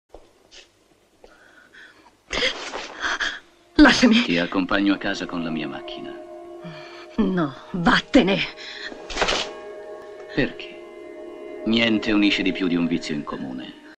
"Lo strano vizio della signora Wardh" di Sergio Martino - Niente unisce più di un vizio in comune
Edwige Fenech appare magnifica nel suo ruolo di donna dominata dalle proprie pulsioni e, contemporaneamente, quasi investita da una sacra missione di autopurificazione rivolta a qualcosa di ben celato. Lei è divisa tra la virtù/trasgressione borghese e il puro travolgimento sensoriale, scevro dall'anticonformismo conformista che imperversa in quegli anni, e che diviene una nuova divisa per coprire la nudità dell'essere.
Jean assume quasi il ruolo di  suo psicanalista libertario, rinforzandone il senso di isolamento sociale e morale, e quindi il proprio potere su di lei.